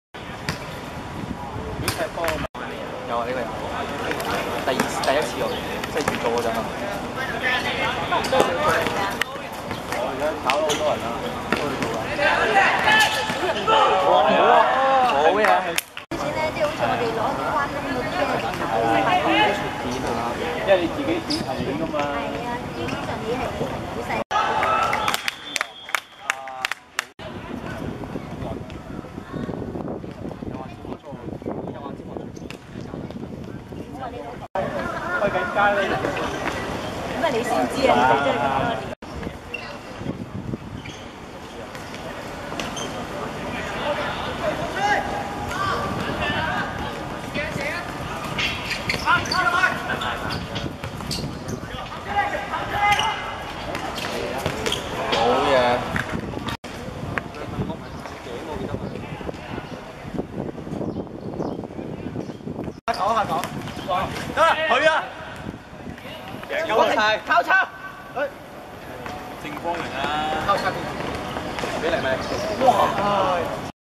安興小飛象 vs JCCSS(東華三院射出燦爛七人足球賽2013準決賽)精華
2013.8.4.天水圍天瑞邨足球塲 18:45 安興小飛象(藍) 1:0 JCCSS(紅)